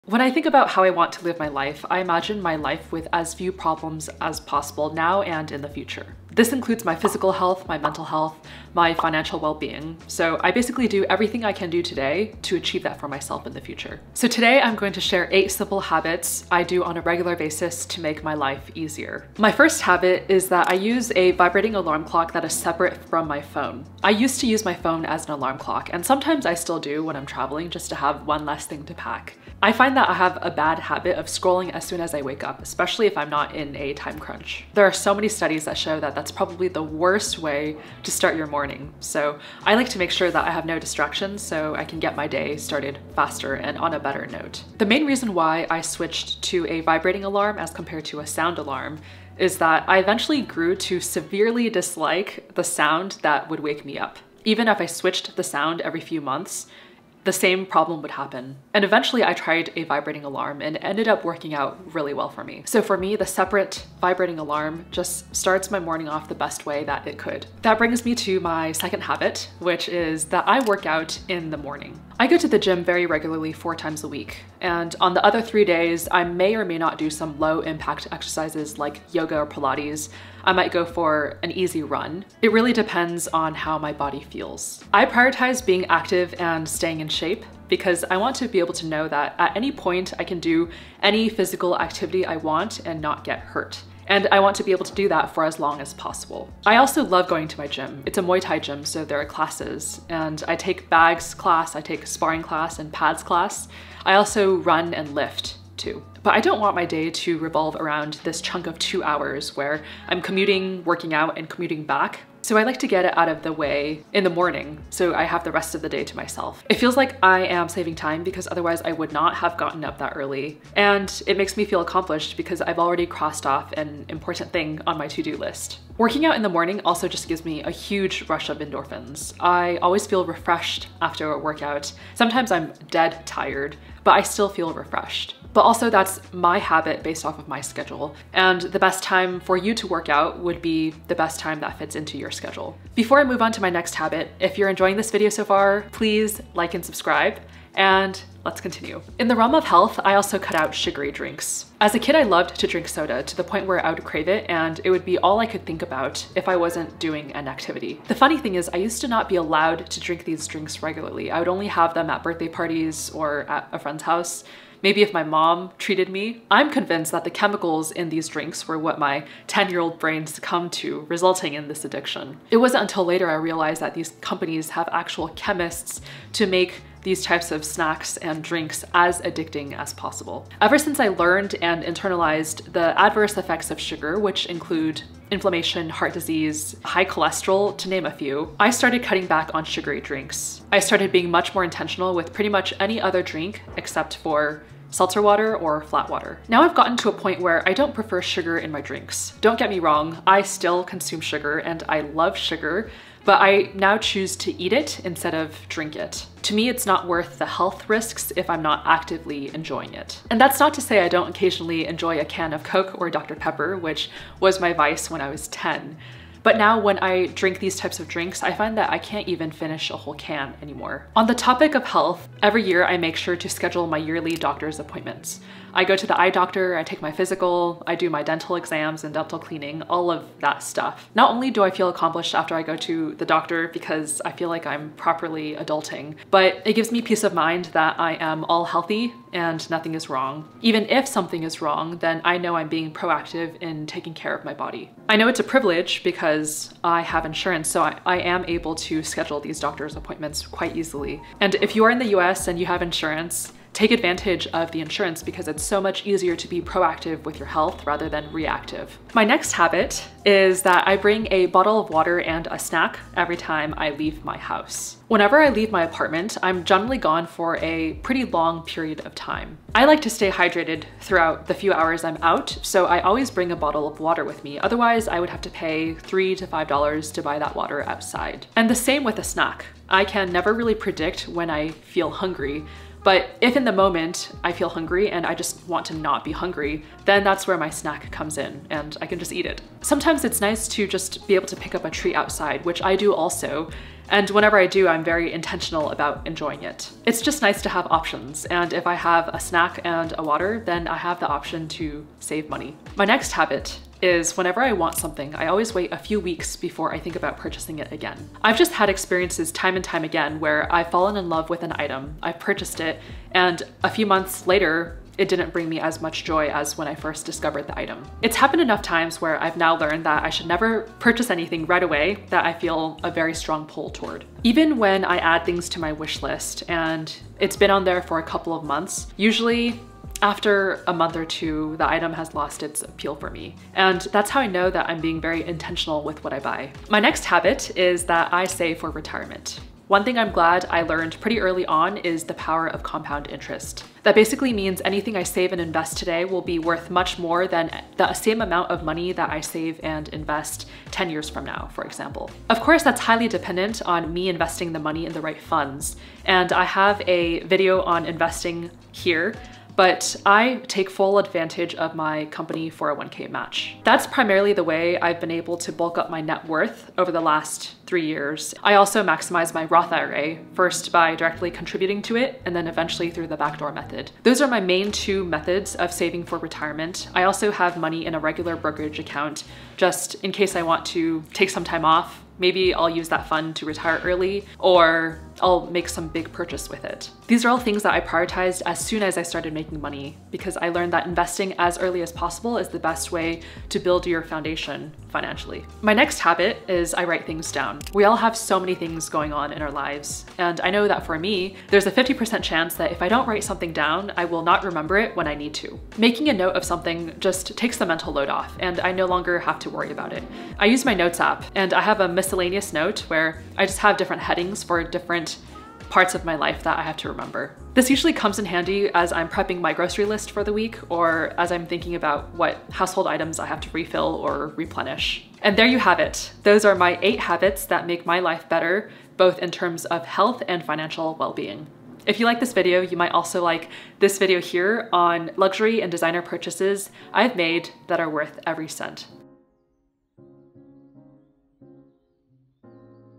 8 Habits to Practice for an Easy Life | Financial, Physical, & Mental Health
Financial, physical, and mental wellness are important for a balanced life. I’m all about setting myself up for the easiest life possible, and today I cover 8 habits I practice to live well. I hope you're able to take some inspiration from these habits to incorporate into your own life.

If you'd like to support me and this channel, consider buying me a coffee!

0:00 - Intro
0:20 - Habit 1
1:10 - Habit 2
2:23 - Habit 3
3:41 - Habit 4
4:19 - Habit 5
5:02 - Habit 6
5:36 - Habit 7
6:35 - Habit 8

Everything I use to create my videos:
Portable SSD disk:
SD Card:
Camera:
Large Tripod:
Small Tripod (for vlogs):